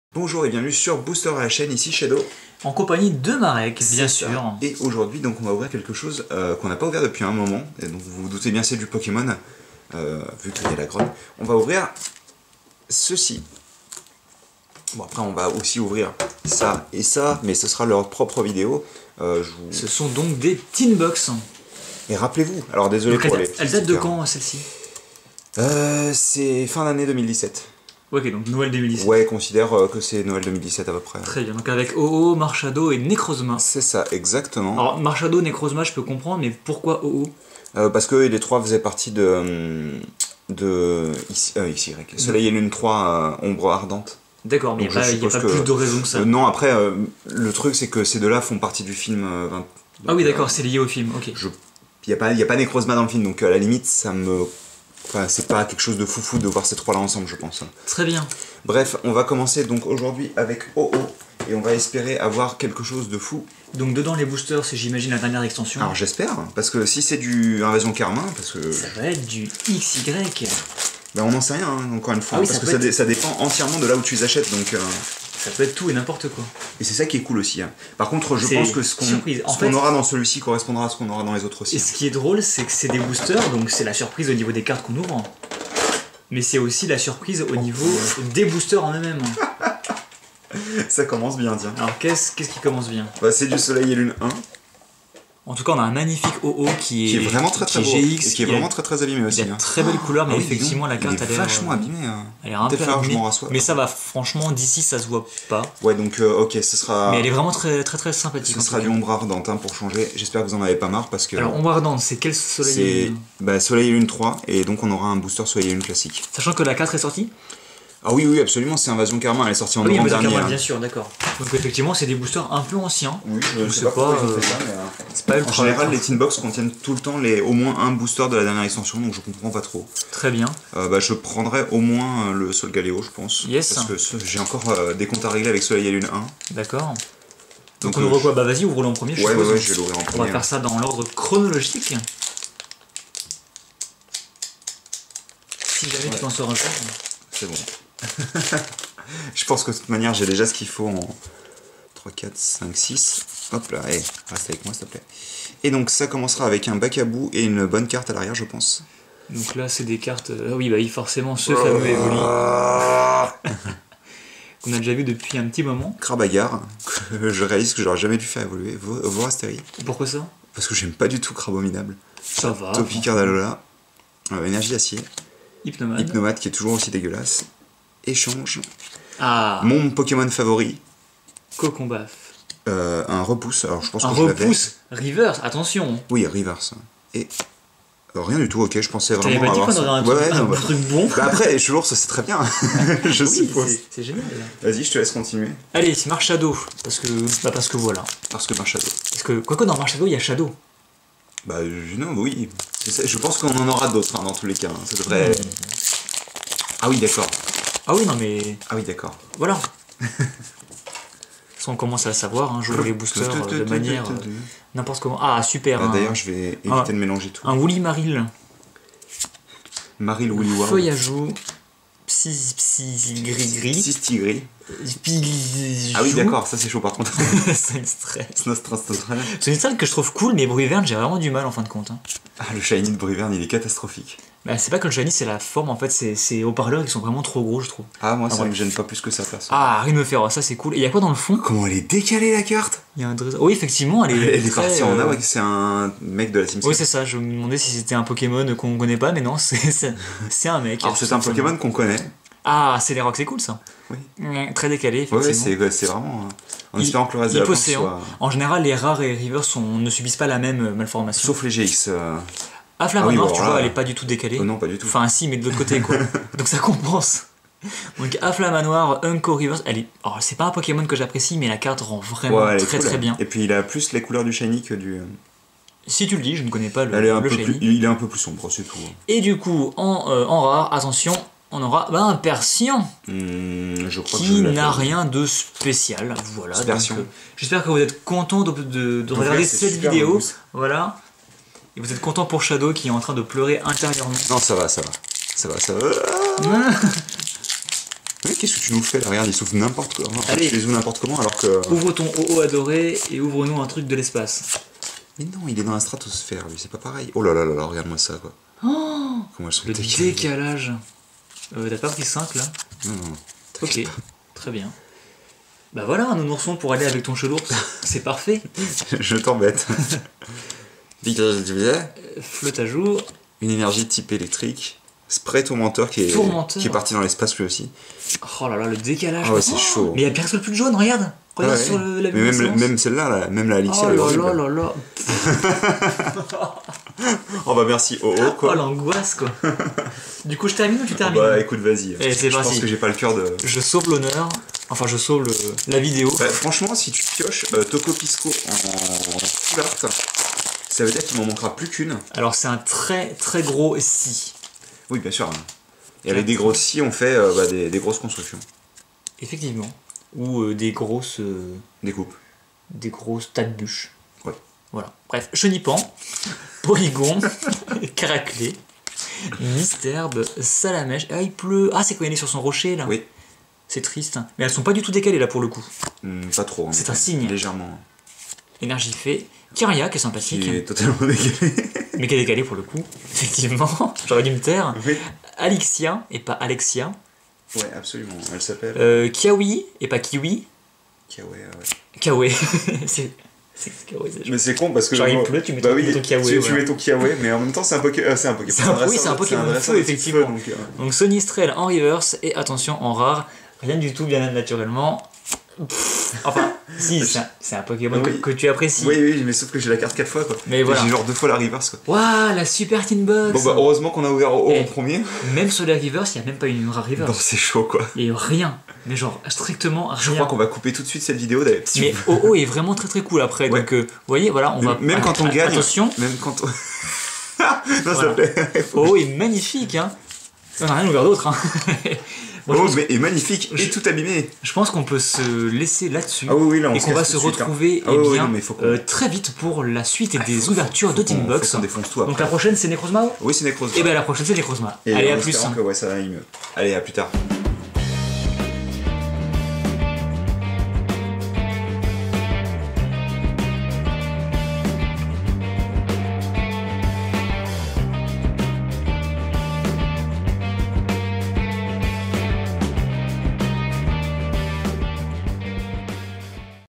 Pokébox Ho-Oh : On se brule les ailes !
Première vidéo de la série des "nouvelles" pokébox, on commence avec Ho-Oh... et il a bien choisi son nom.